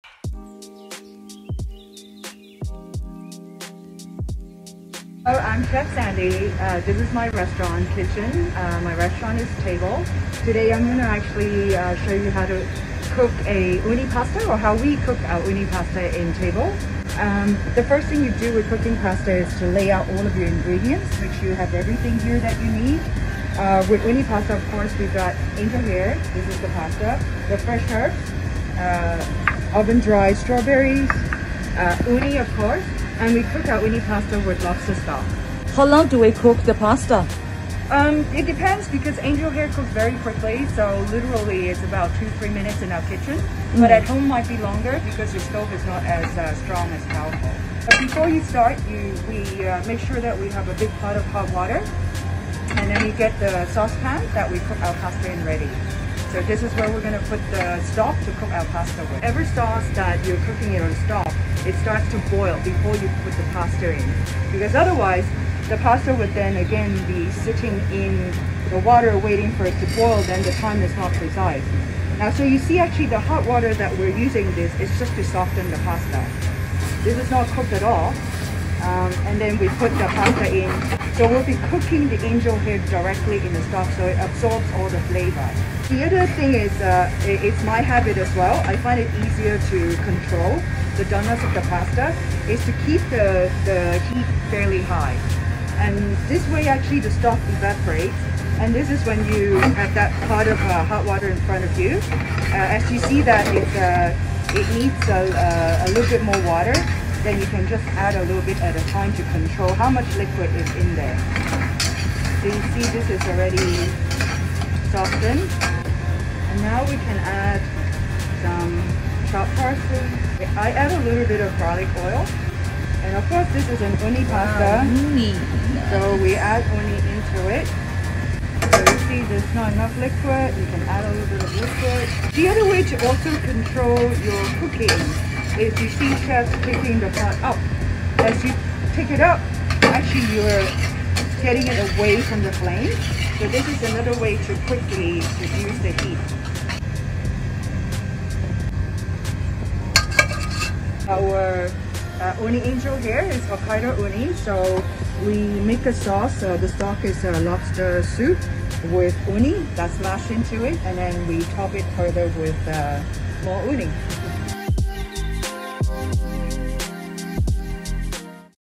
Professional Chef Tips with Chef Sandy Keung
Chef Sandy Keung from Table by Sandy Keung show is how to prepare her customer's favorite Uni Lobster Bisque Angel Hair Pasta